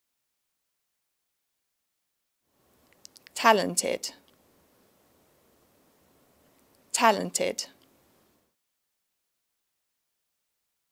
How to pronounce TALENTED in British English
This video shows you how to pronounce TALENTED in British English. Speaker has an accent from Kent, England. Collins Dictionary, the home of living English and pioneers of dictionary publishing